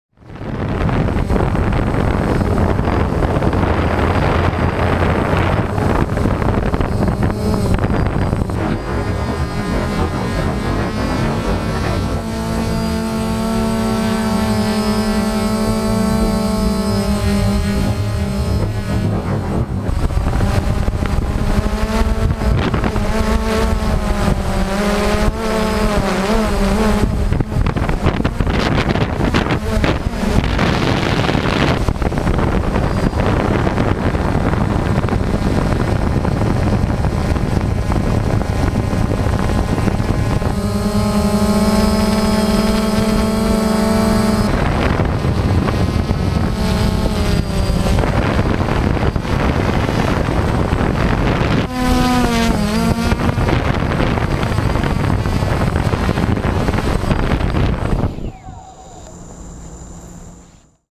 First 2-axis Gimbal test
Zenmuse for Phantom arrived!
ProTune 1080
NoFisheye
First test...